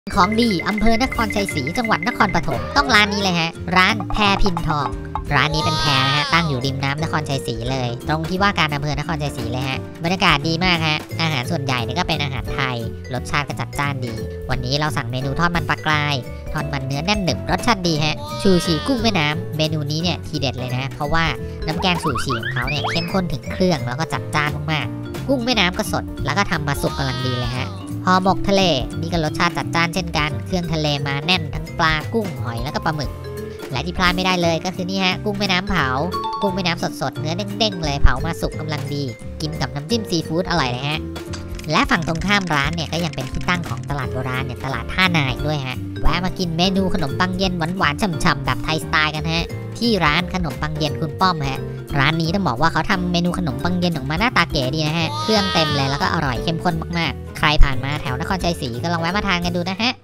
ร้านเด็ดนครชัยศรี แพพิณทอง อาหารไทยเรือนแพริมน้ำรสจัดจ้าน
ร้านเด็ดนครชัยศรี แพพิณทอง อาหารไทยเรือนแพริมน้ำรสจัดจ้าน ใครผ่านไปเที่ยวนครปฐมแนะนำเลย ร้านนี้บรรยากาศดี อารหารรสเด็ด ราคาโอเคเลย